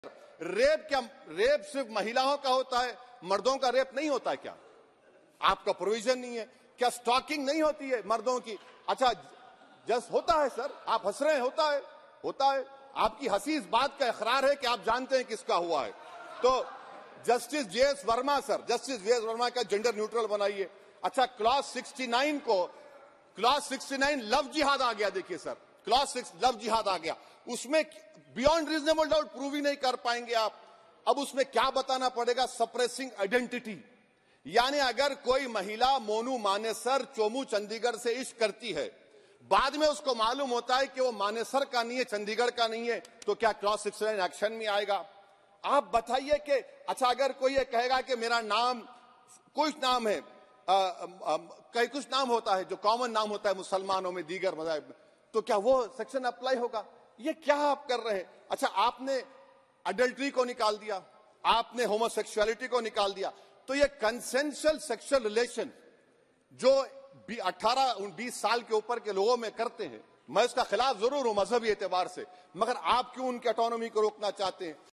Asaduddin Owaisi On New Criminal Laws: "Don't Men Get Raped, Stalked?"
AIMIM chief Asaduddin Owaisi said even men are raped and stalked, but there's no provision to protect men from such offences in the new criminal laws. His statement came in the Lok Sabha during a debate on the new criminal laws that have now cleared by the parliament.